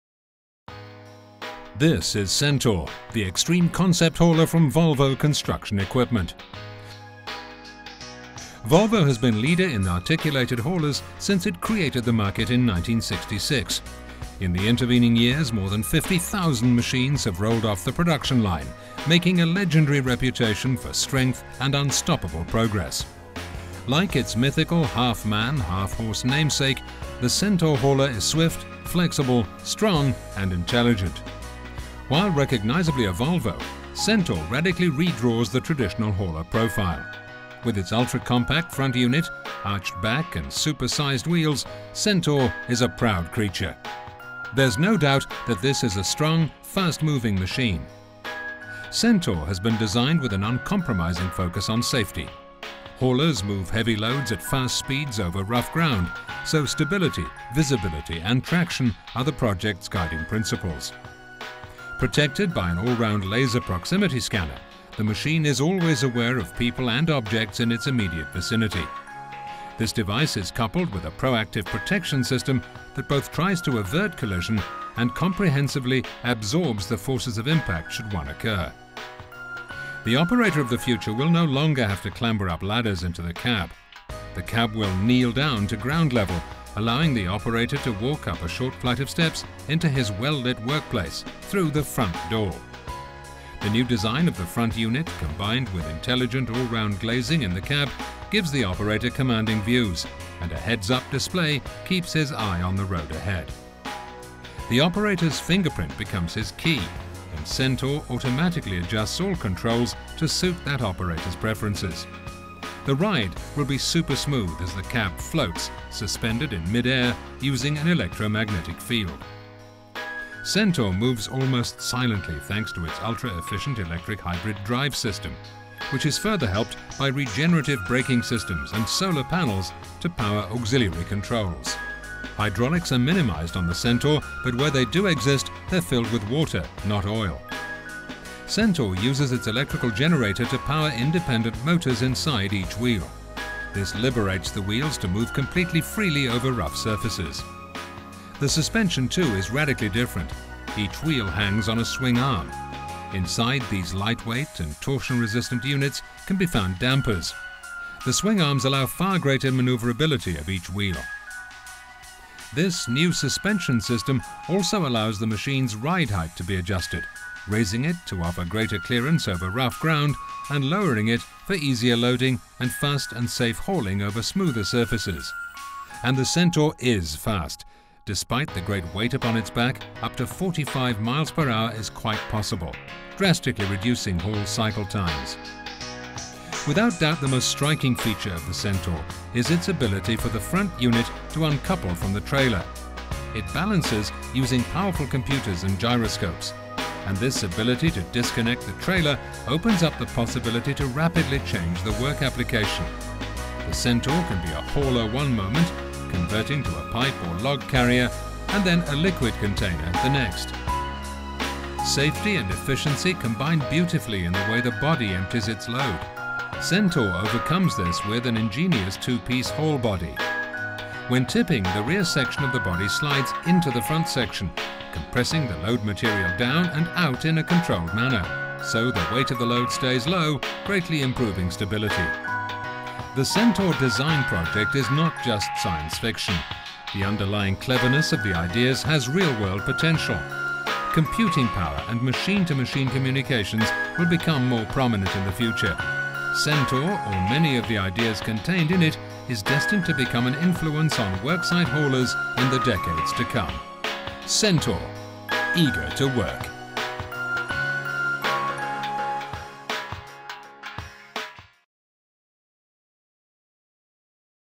Volvo CENTAUR - Volvo's futuristic Dumper concept machine
Presentation of the CENTAUR, possible Volvo evolution of the construction equipment (here a dumper) in the years to come.